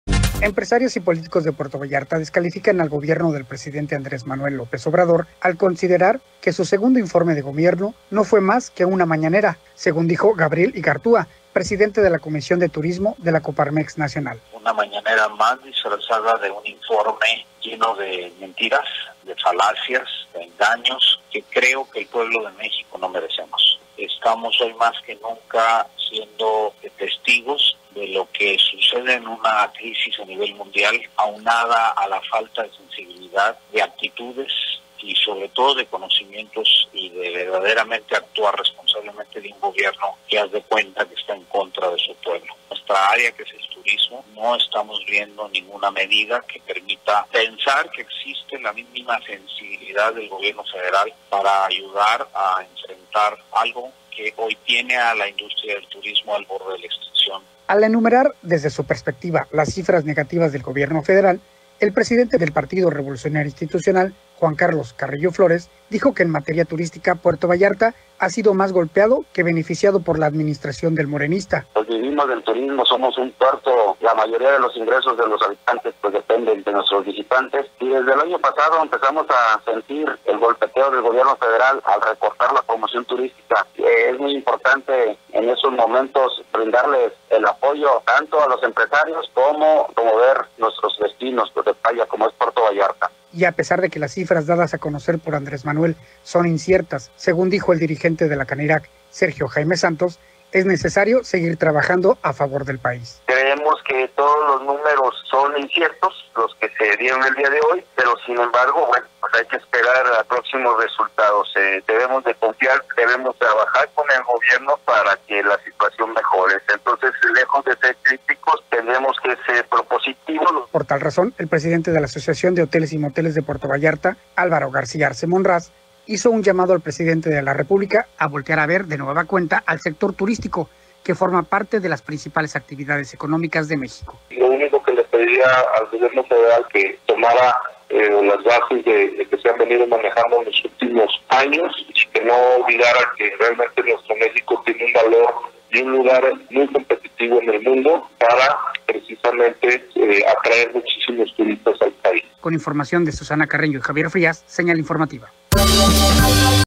El gobierno de Andrés Manuel López Obrador le ha salido a deber a Puerto Vallarta
El gobierno de Andrés Manuel López Obrador le ha salido a deber a Puerto Vallarta, coinciden empresarios y políticos vallartenses